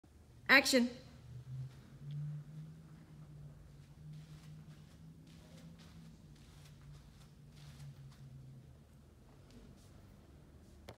KB Bottoms Up Lateral Waiter Walk
1- Keep elbow up with shoulder
2 - Hold ribs down with opposite hand
3 - Push off back foot to step